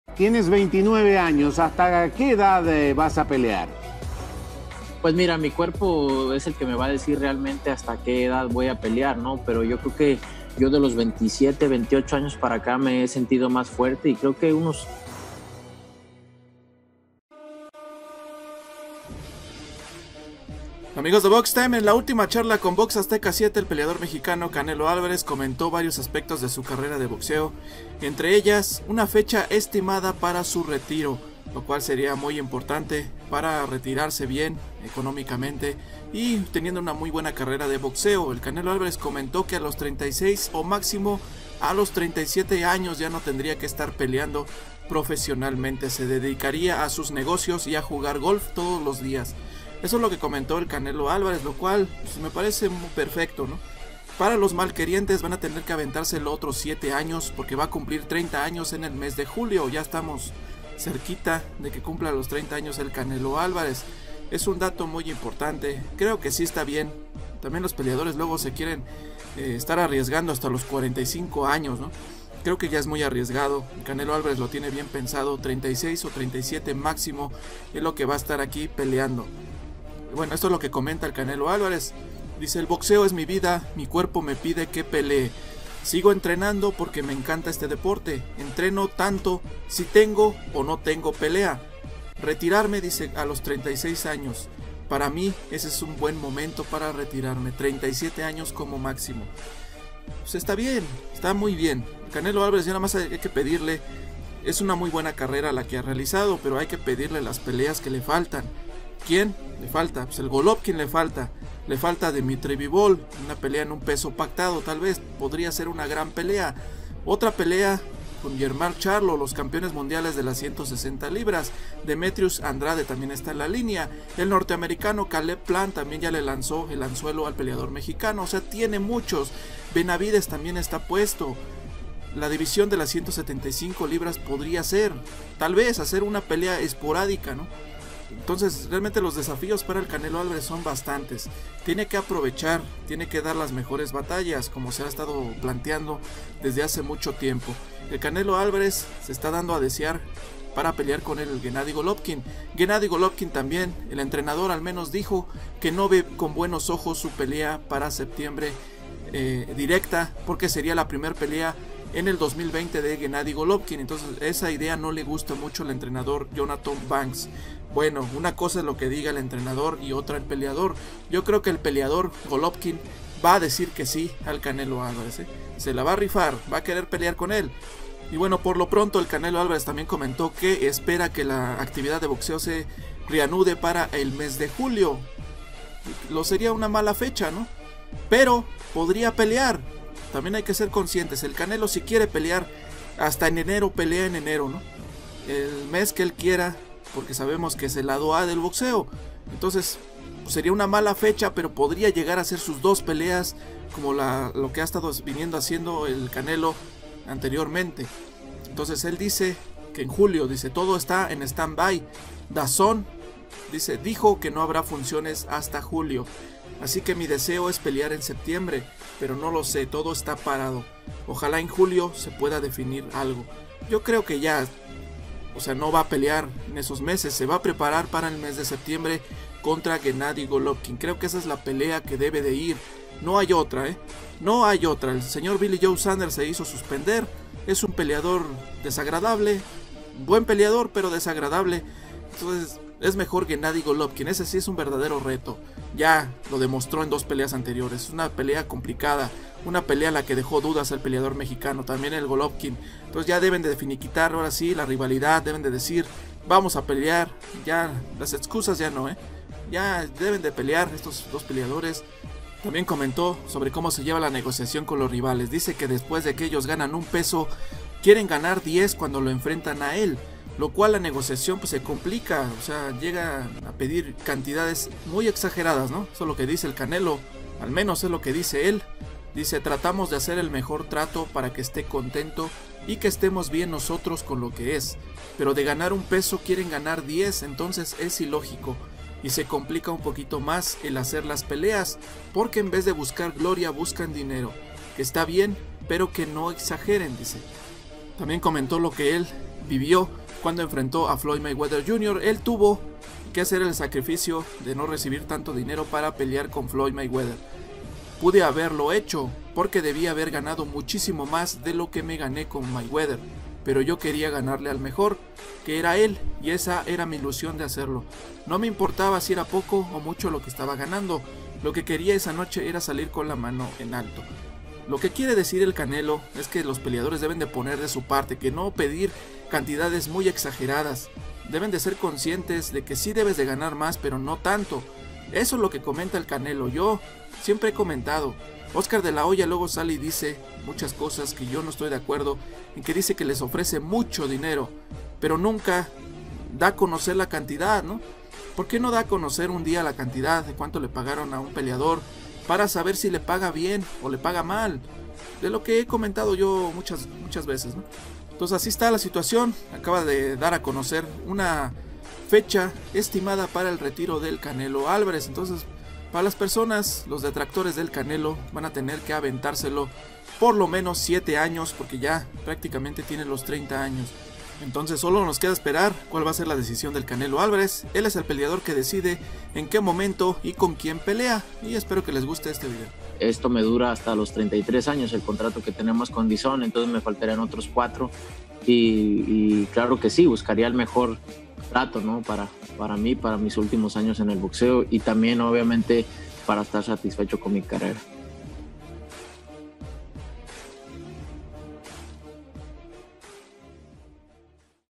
CANELO ALVAREZ REVELA CUANDO SE RETIRA DEL BOXEO
El peleador mexicano Canelo Alvarez revela cuando se retira del boxeo y que es lo que espera en el futuro